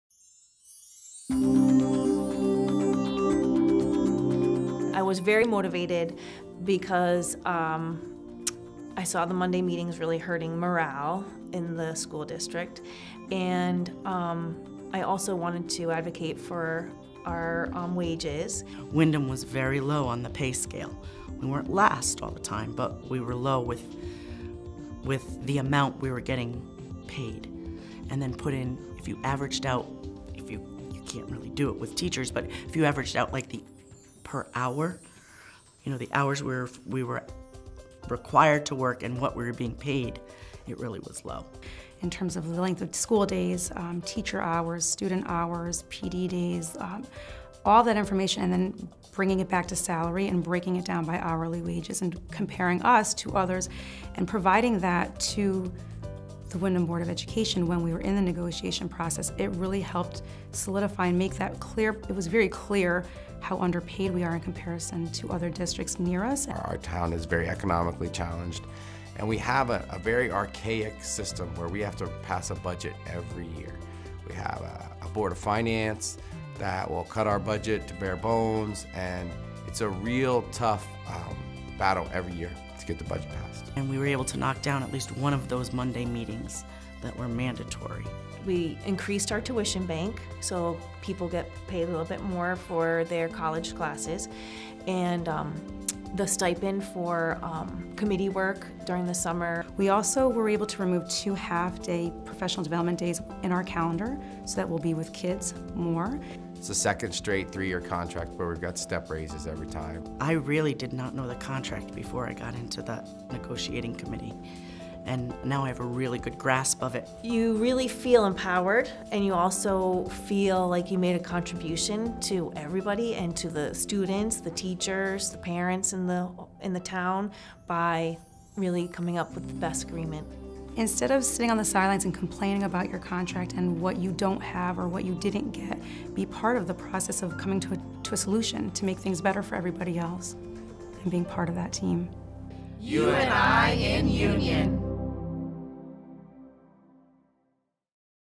U & I in Union: Negotiating a Better Future
Watch three PreK-12 teachers and their local union president share how they negotiated a new contract with school officials that achieved long-overdue parity for the district's educators.